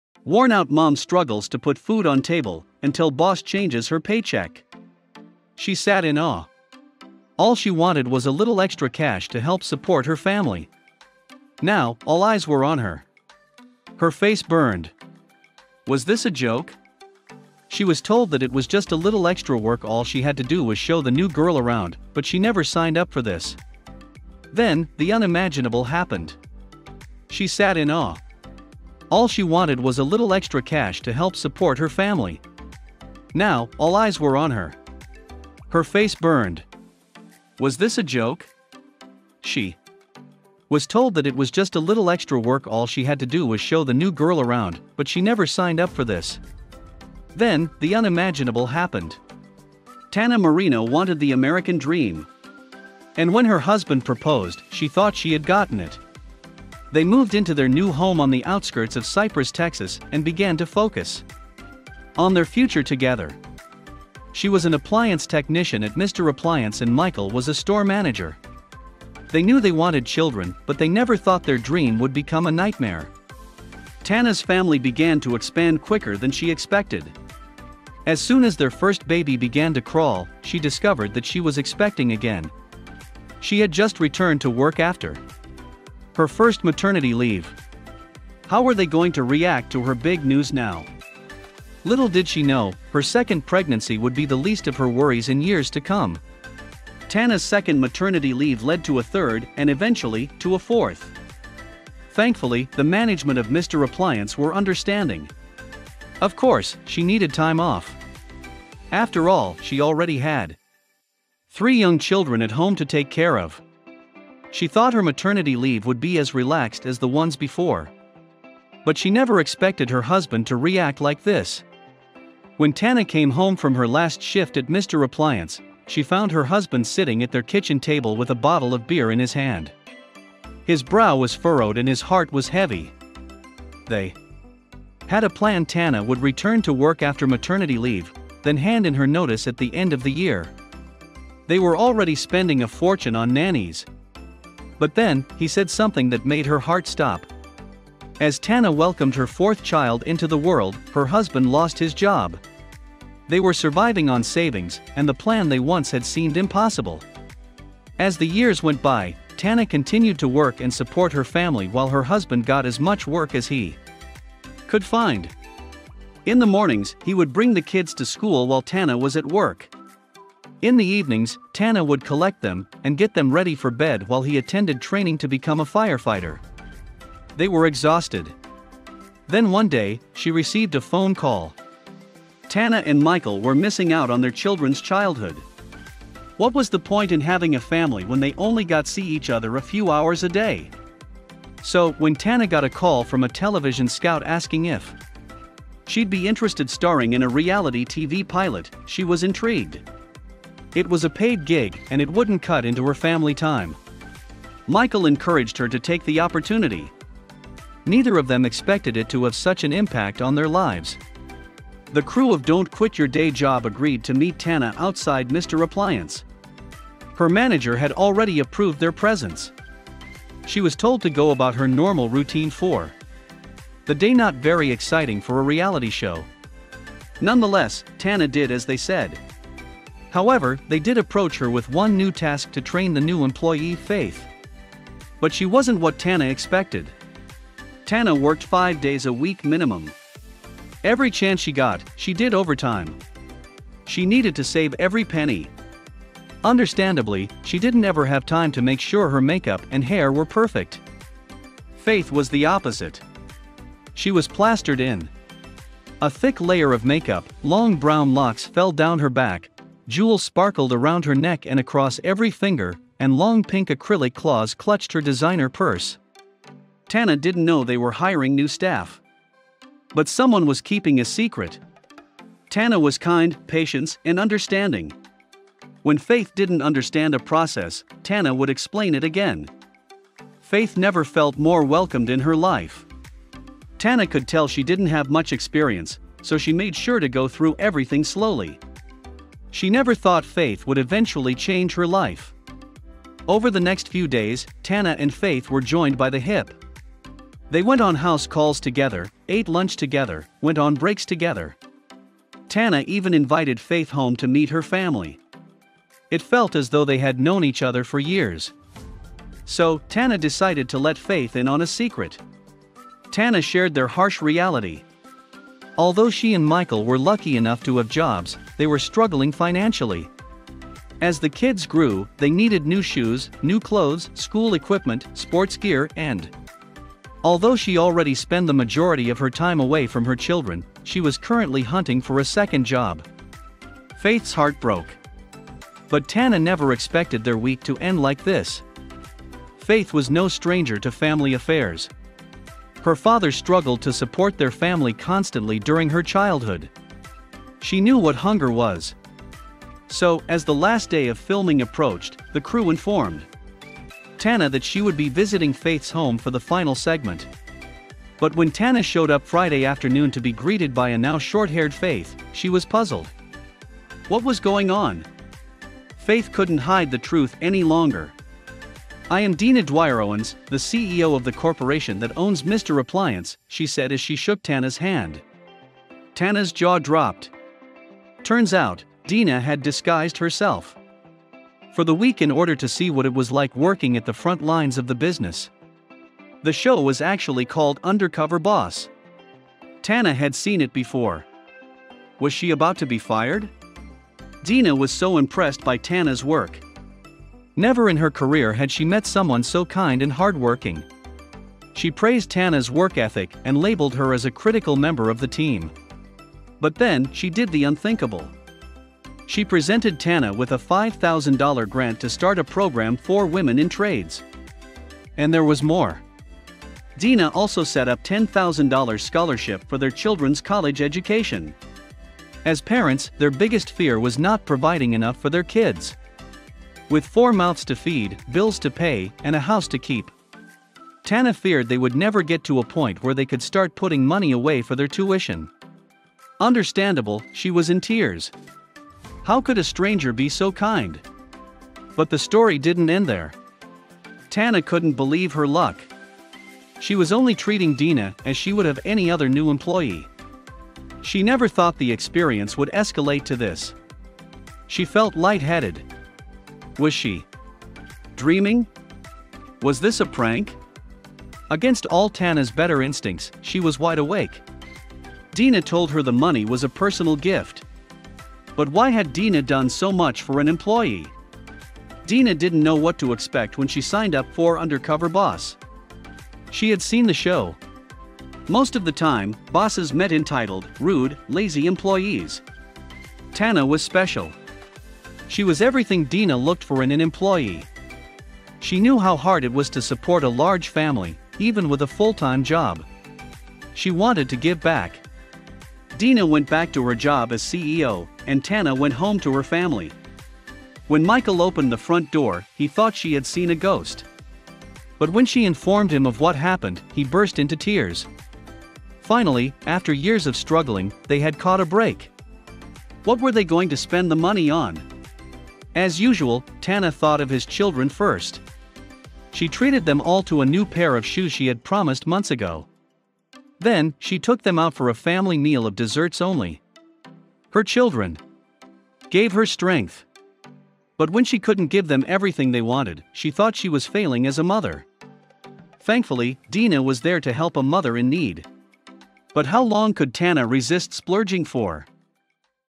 Worn Out Mom Struggles To Put Food On Table, Until Boss Changes Her Paycheck
Tanna Marino, a married mom with four kids, was having trouble making ends meet, working as an appliance technician. Then Dina Dwyer-Owens, the boss at her company confronted her.

Not Sure What To Expect
Marino was working for a company called Mr. Appliance, a franchise of the Dwyer Group, an organization that provides repair and maintenance services. In 2011 she got caught up in a situation, and her life would never be the same.

Don’t Quit Your Day Job
The would-be TV star agreed to appear in a show entitled Don’t Quit Your Day Job. However, it turned out that the program was actually an episode of the CBS program, Undercover Boss.

In this series, a top-level manager works anonymously with employees at the bottom of their company. In doing so, they learn all about their employees.